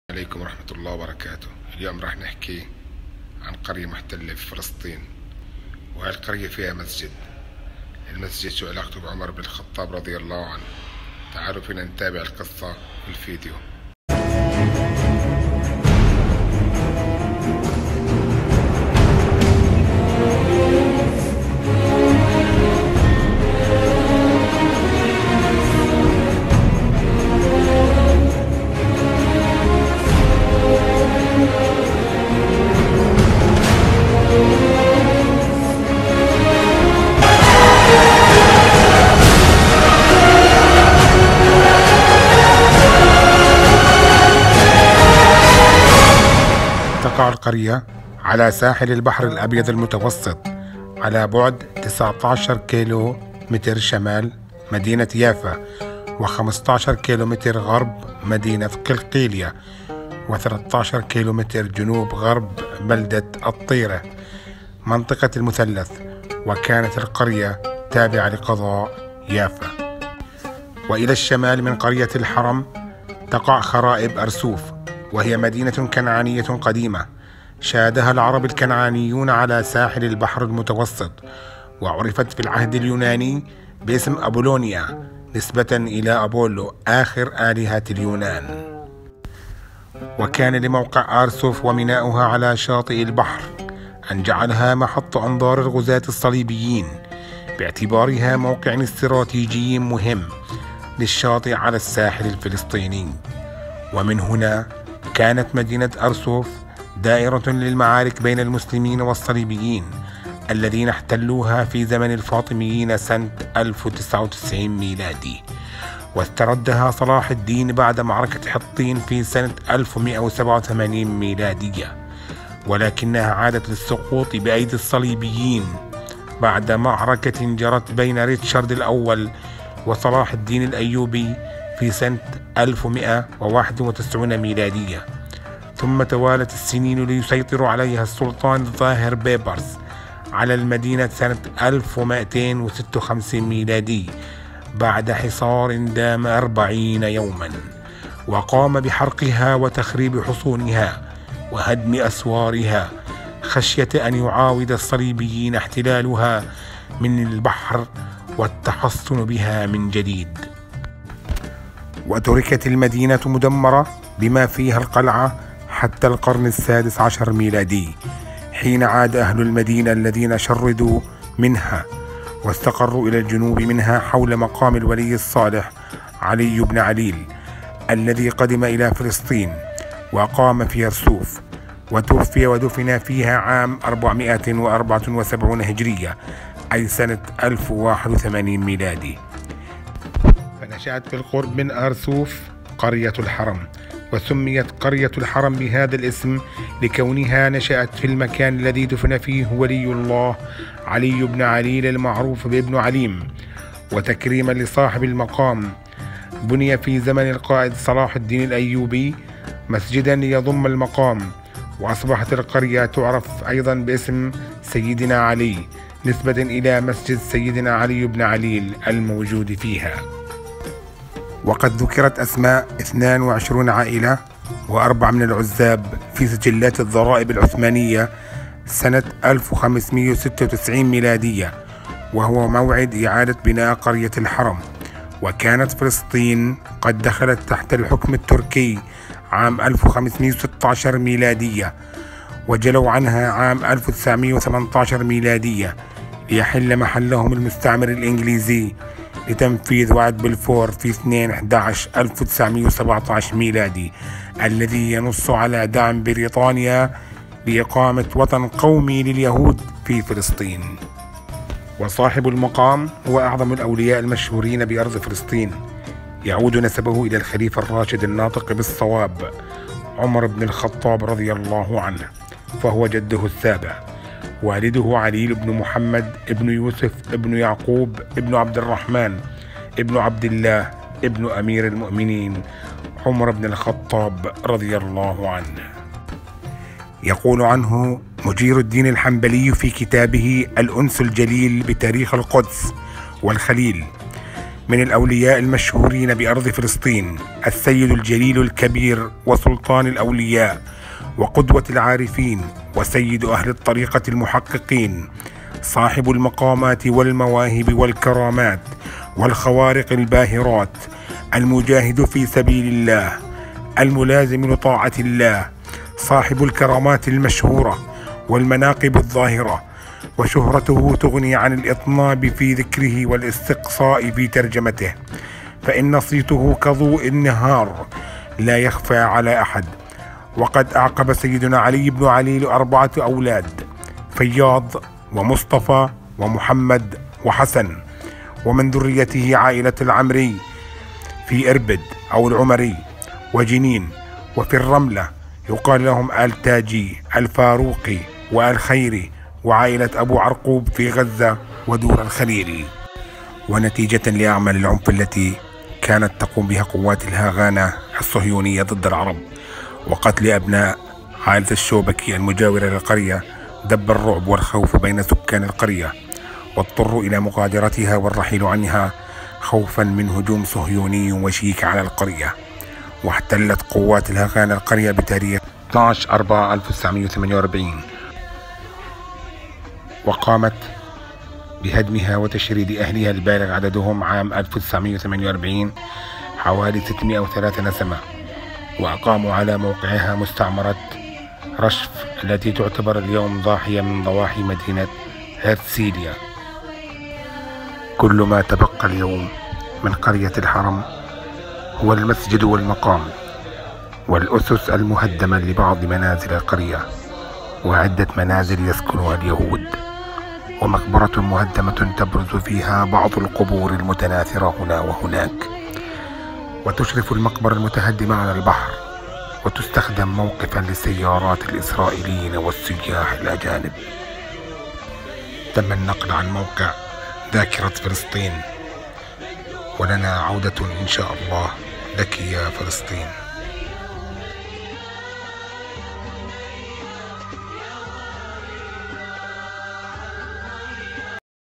قرية الحرم ومسجد سيدنا علي مدينة هرتسيليا الاسرائيلية
مسجد سيدنا علي و هو مسجد قرية الحرم المهجرة المقام اليوم فوقها مدينة هرتسليا الإسرائيلية.